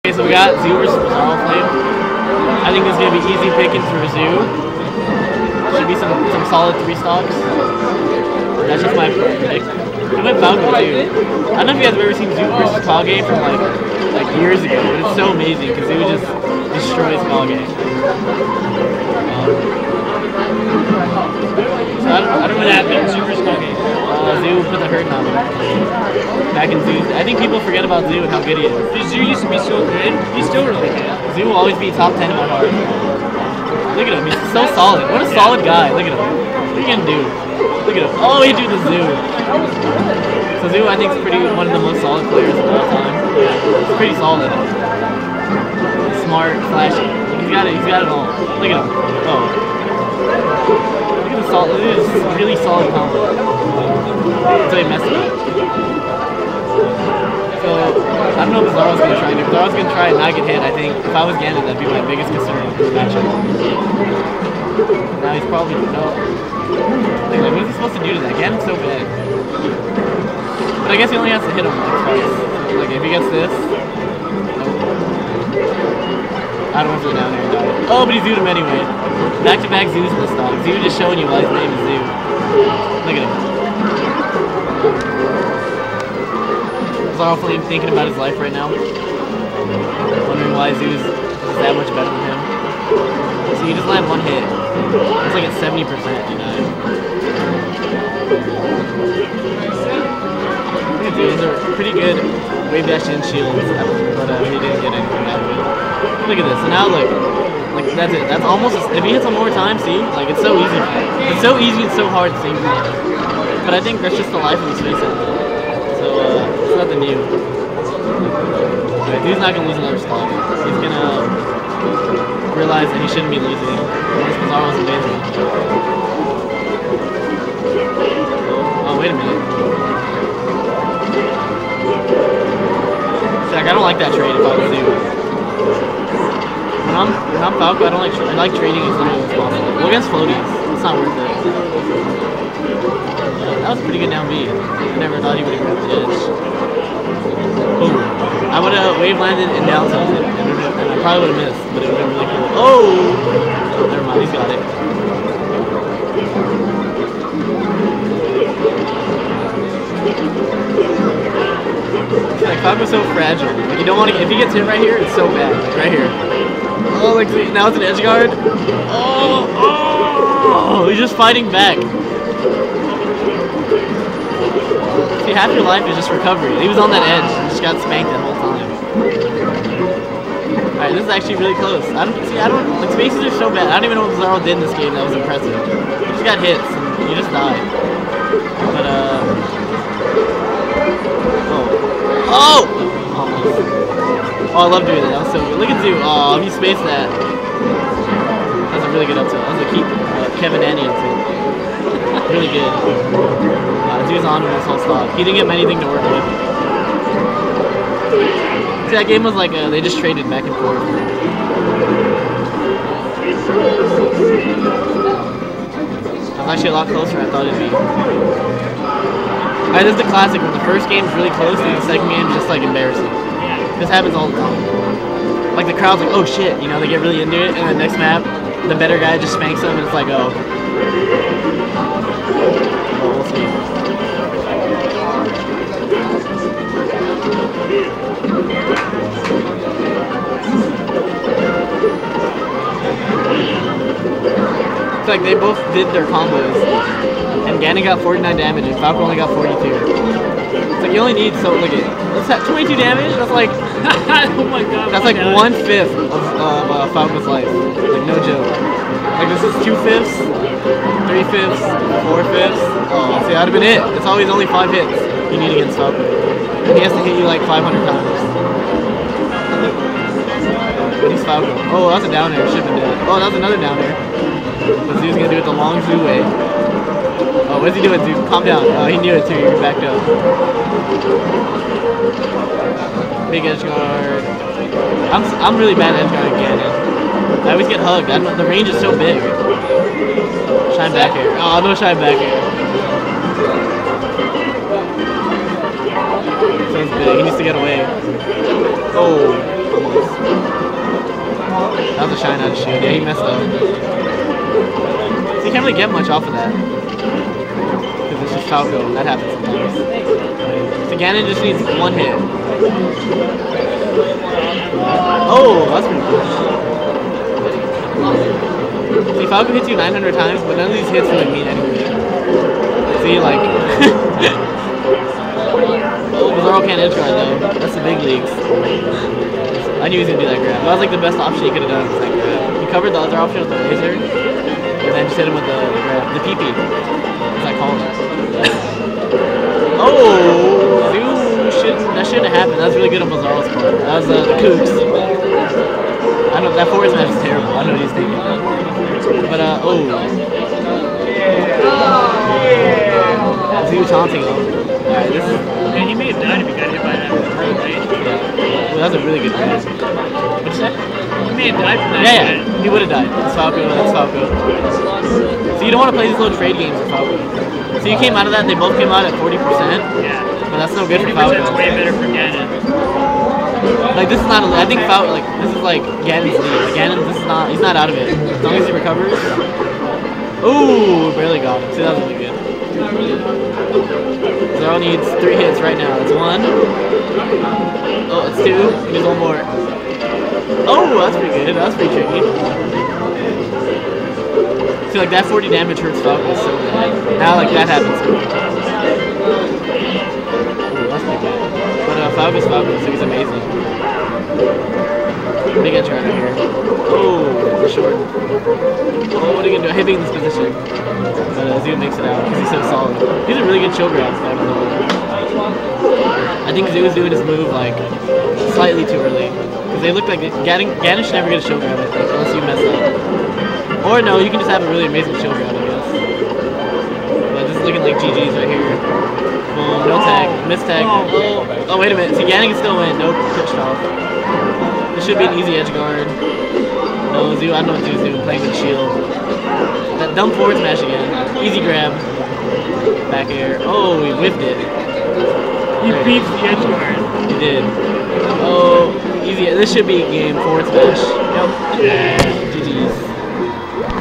Ranbat Singles WQ: Zhu (falco) vs BizzarroFlame (ganon)
Tournament: Ranbats
Date: 22 Feb 2013
Location: Oakland, CA
Uploader: JB

Commentary: mango and scar